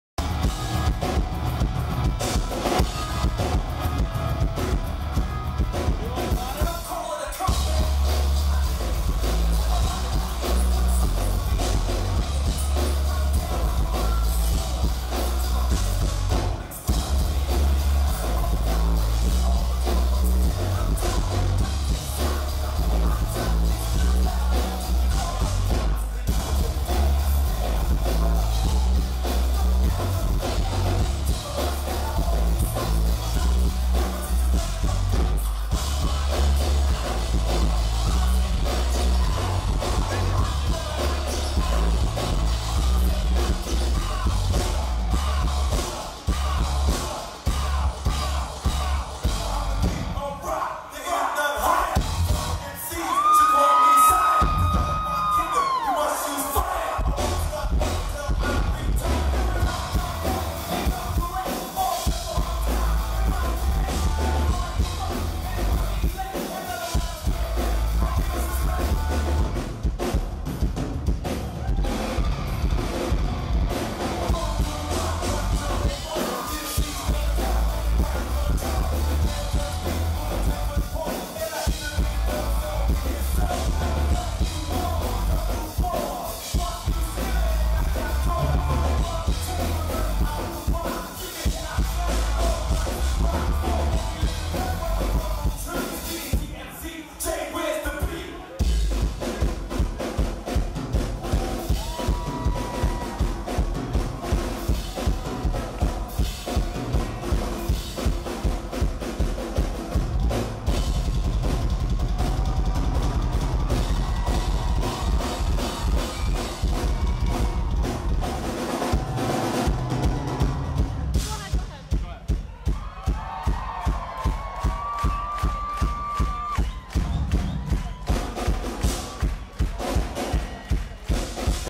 Sum 41's Frank Zummo's Drum Solo LIVE (HD) @ Phoenix Concert Theatre, Toronto
Drum Solo
Don't Call It a Sum-Back Tour
October 18, 2016
Phoenix Concert Theatre,